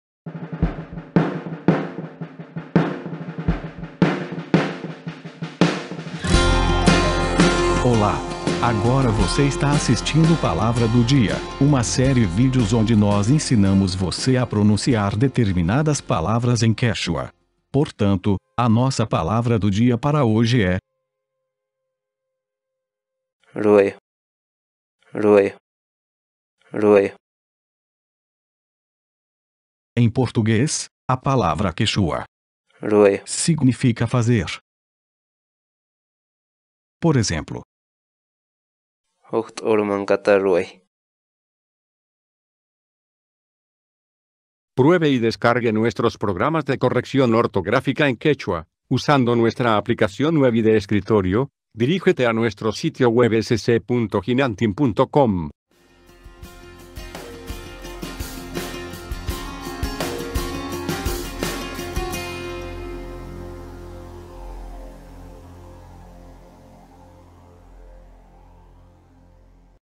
Como pronunciar rap'i ‘hoja’
Social

Grupo de Investigación Hinantin: La entrada ‘rap'i’ es parte de la base para el desarrollo de un diccionario hablado disponible gratuitamente para el idioma quechua.